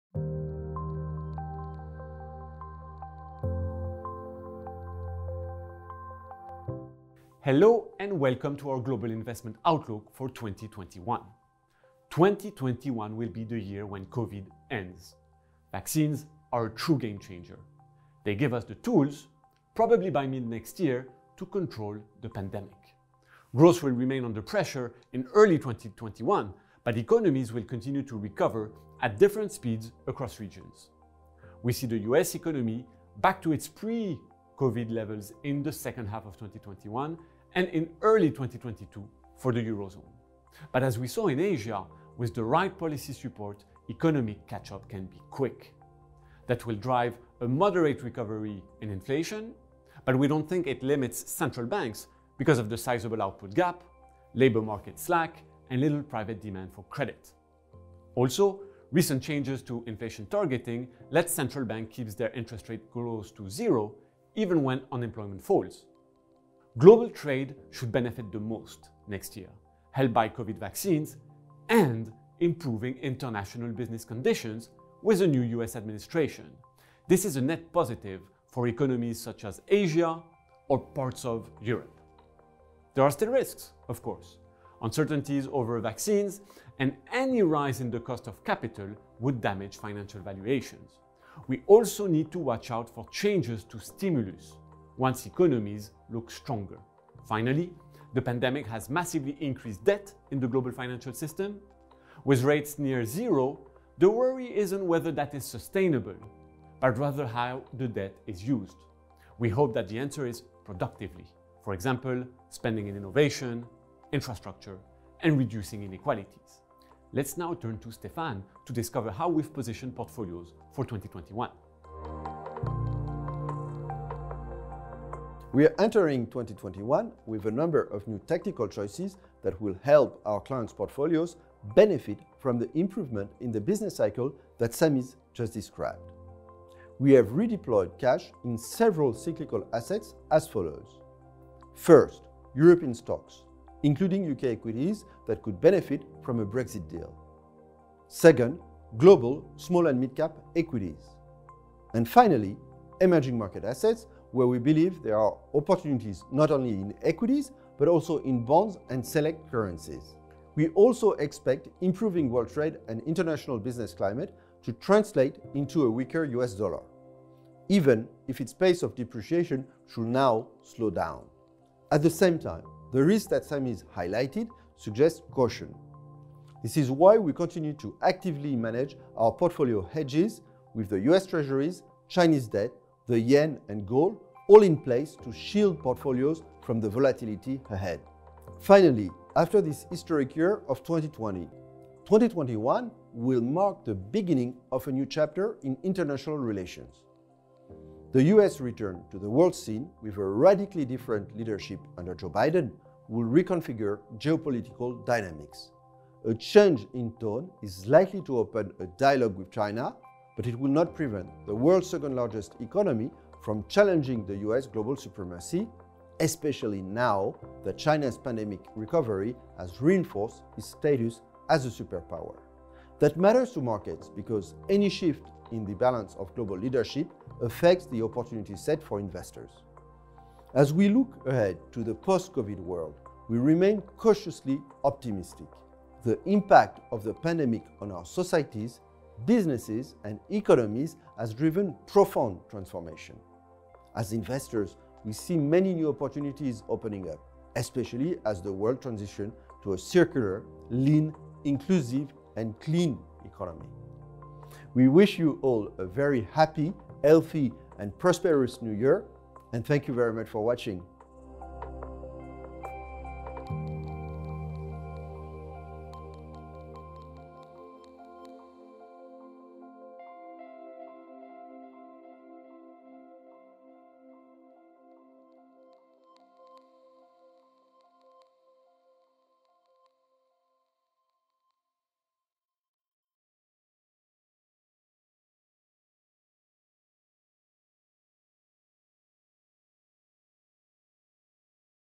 Global Investment Outlook 2021
Thanks to COVID vaccines & better international business conditions, economic catch up is set to continue in 2021. But not without risks.